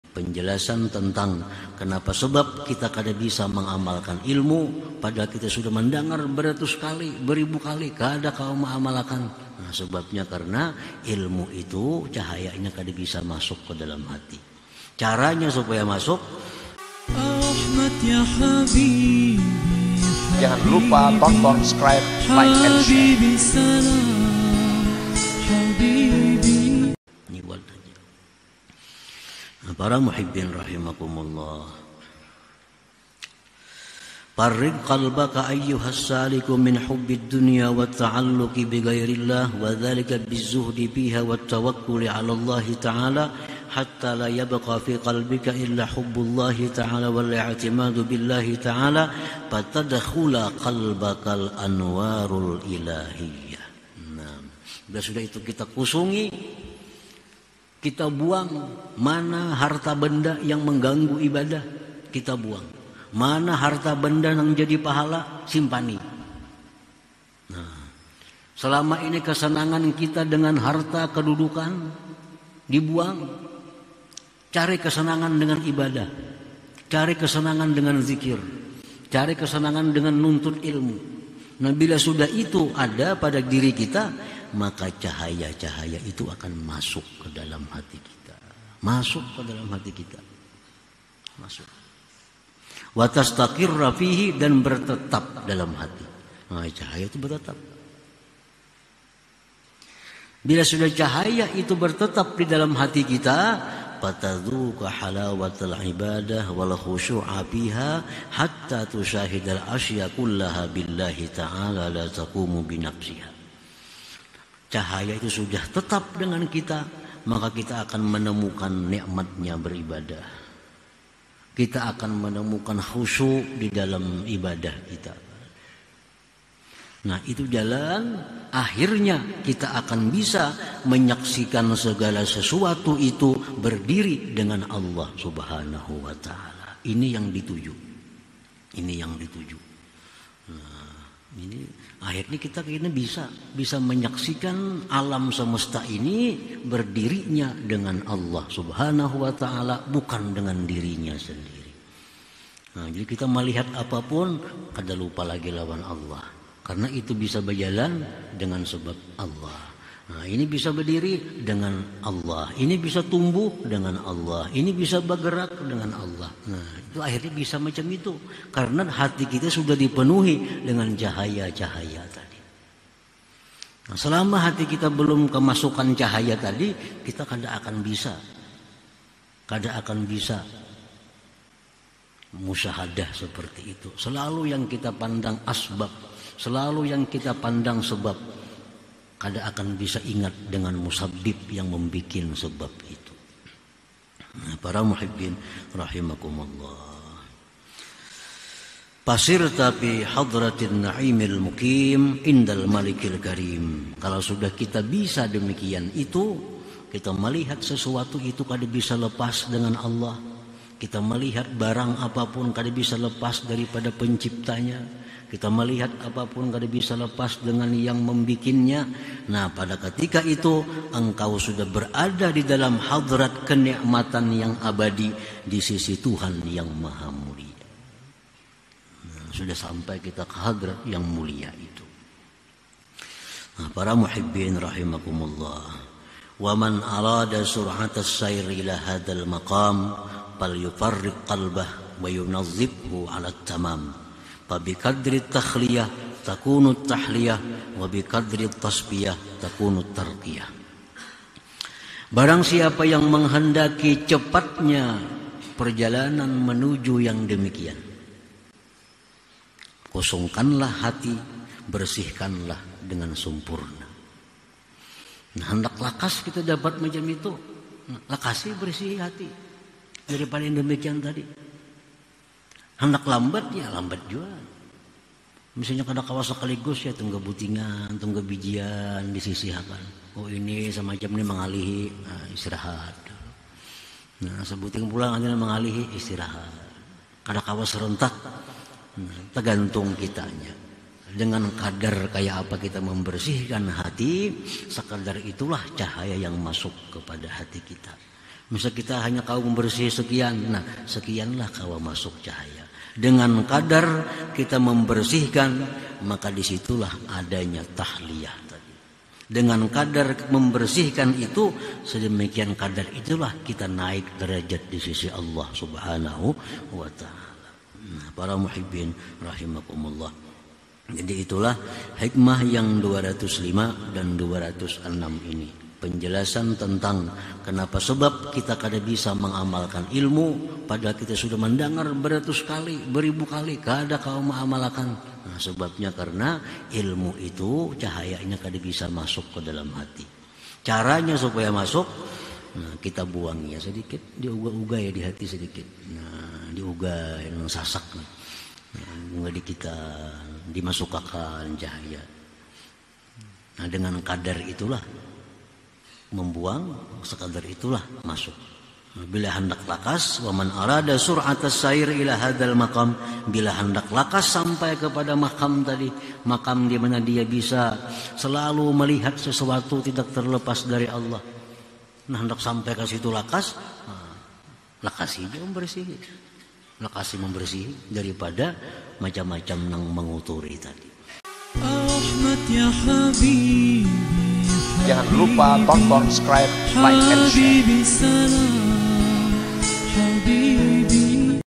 Penyebab cahaya Ilmu tidak bisa masuk kedalam Hati
Kajian TareKat Hakekat Ma'rifat Tasauf
Jalan Mengenal  Allah dan Rasulullah
Agar mendapat  iklas yang sempurna
kunci hidayah adalah taubat